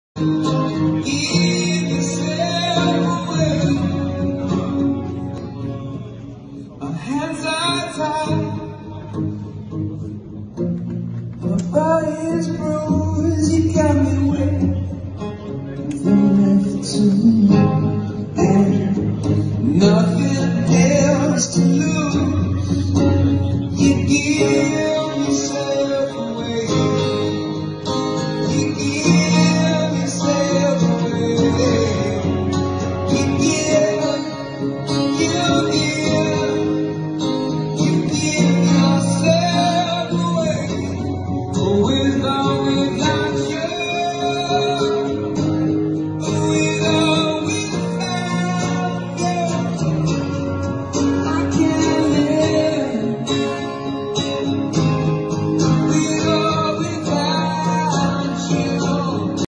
Bono singing in Kyiv bomb shelter
Bono singing in Kyiv bomb shelter. Khreshchatyk subway station in Kyiv.